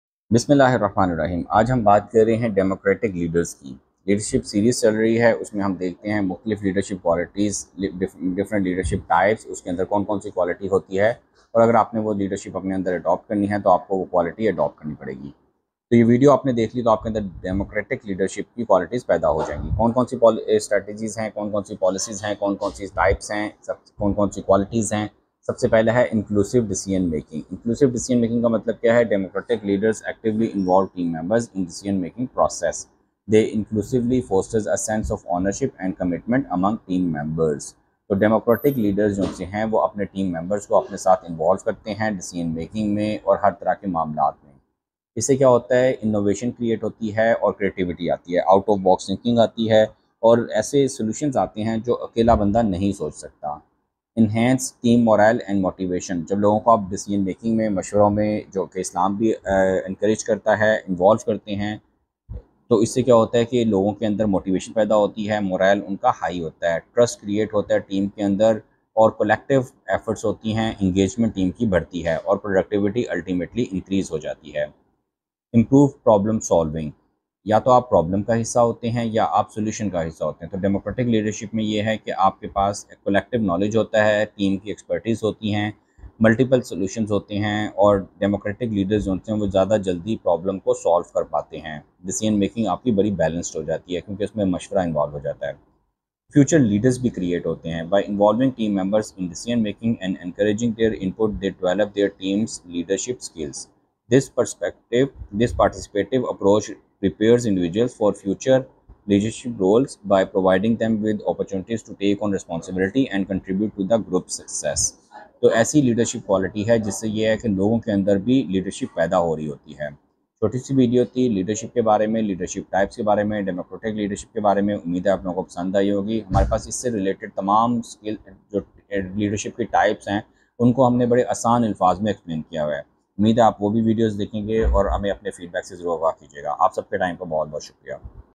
Democratic Leadership : Empowering Teams Through Inclusive Decision-Making | Leadership Styles
Unlock the power of Democratic Leadership, where team collaboration and inclusive decision-making drive success. In this video, we explore how democratic leaders engage their teams, foster creativity, and build strong, motivated workforces. Perfect for managers, entrepreneurs, and leaders in Pakistan and India looking to adopt a leadership style that values input and encourages shared responsibility. Learn the benefits and challenges of Democratic Leadership and how it can transform your organization in 2024. Don’t miss this essential guide to creating a more inclusive and innovative workplace!